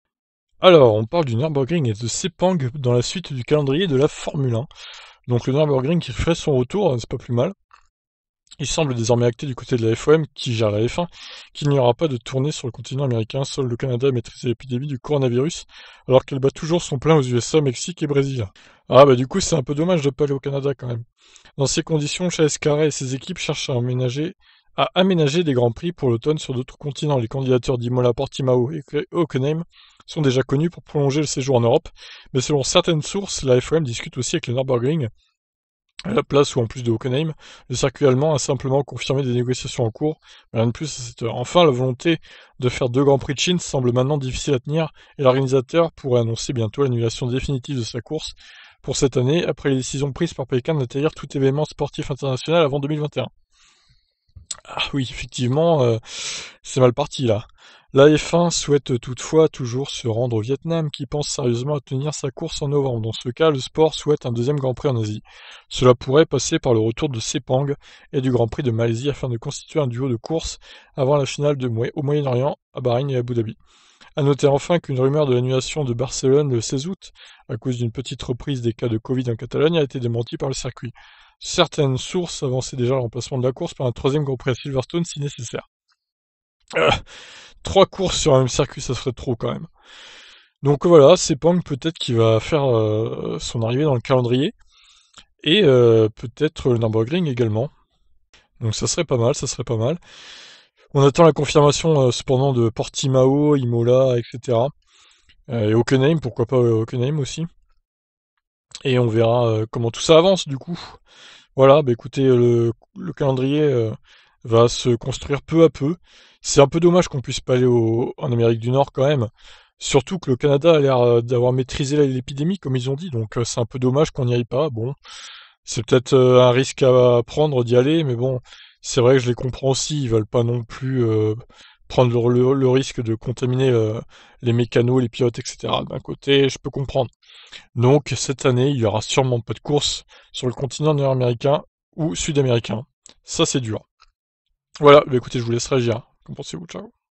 Le Nurburgring et Sepang également ajoutés au calendrier ?
On se dirige vers 0 course en Amérique cette saison !